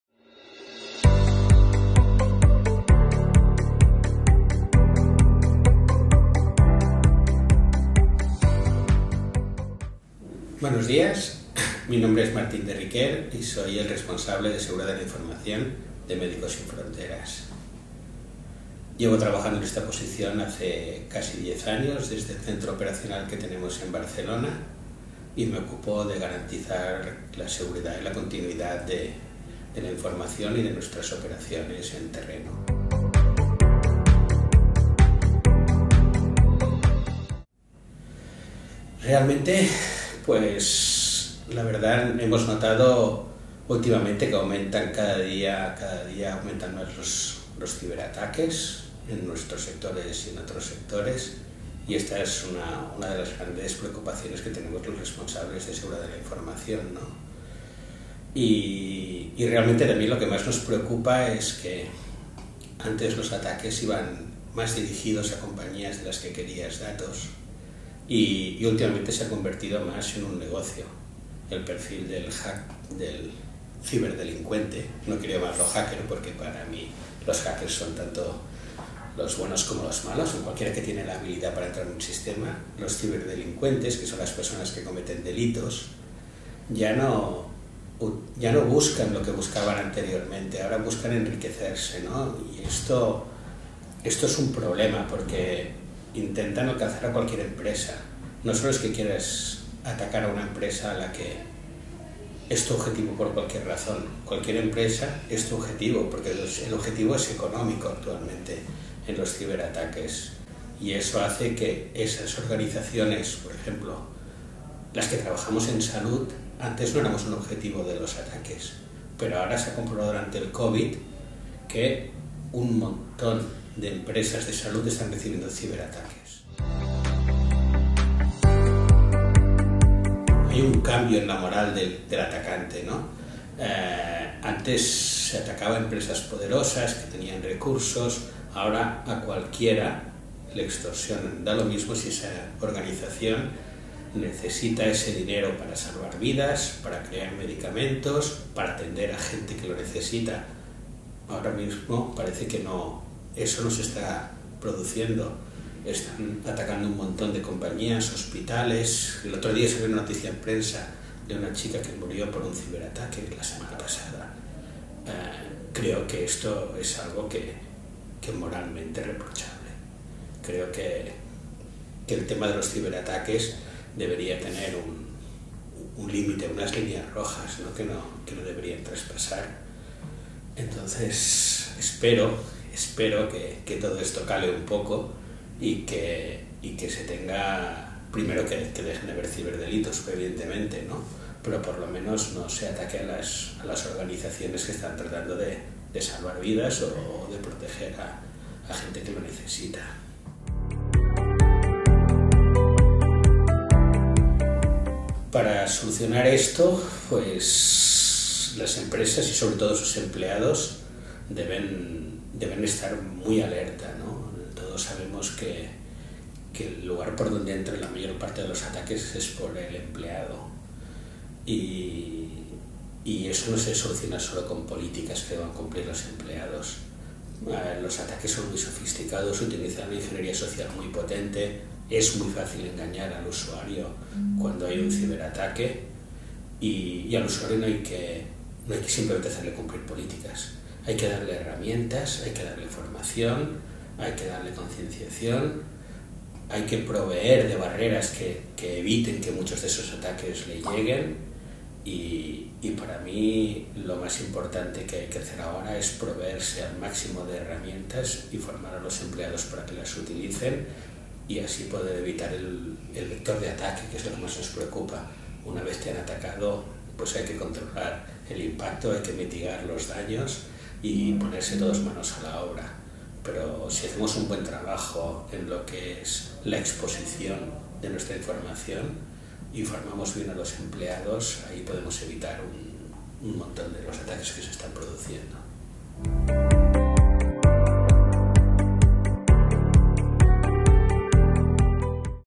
Programa Concienciación Tranxfer | Café con el experto Martín de Riquer Mestres
Head of Information Security en Médicos Sin Fronteras (MSF)
Líder experimentado en Seguridad de la Información y protección de datos con más de 30 años de experiencia laboral y más de 20 dedicados a la seguridad de la información y la protección de datos.